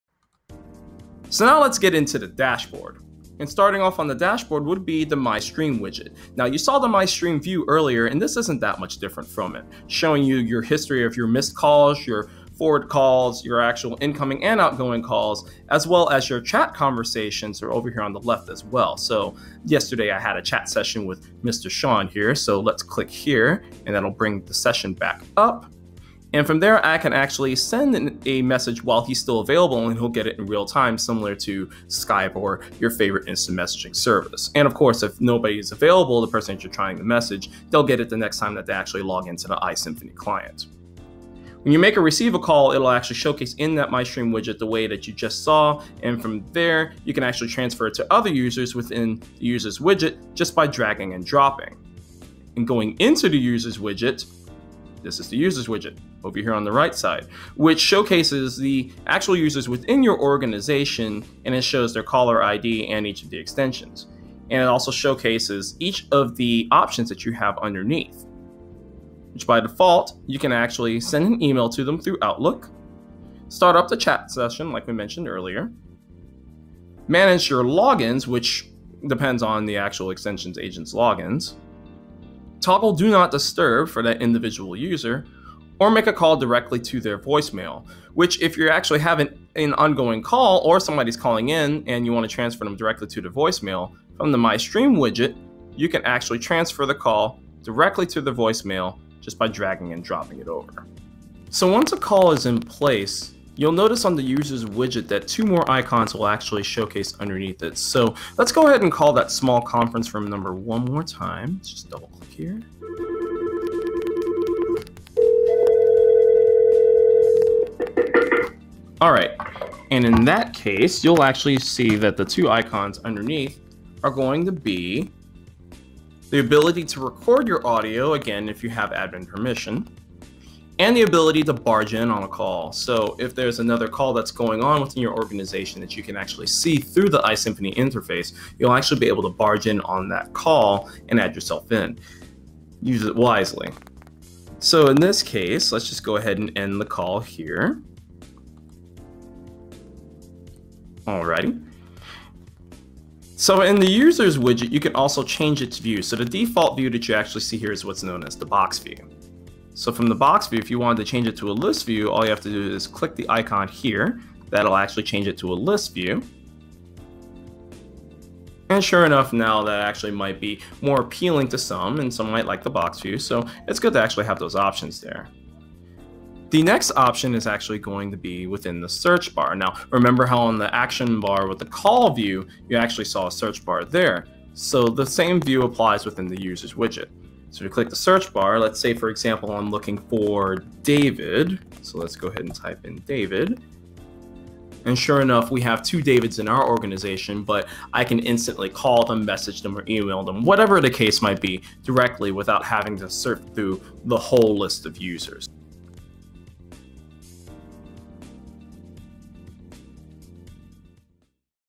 iSymphony Dashboard Training Part 3 - My Stream & Users Widgets - neoRhino IT Solutions
This is part 3 of the training video we made for the iSymphony Dashboard which covers the My Stream Widget & Users Widget.

If you're in the Houston, TX area and want to know more about the services we offer, visit our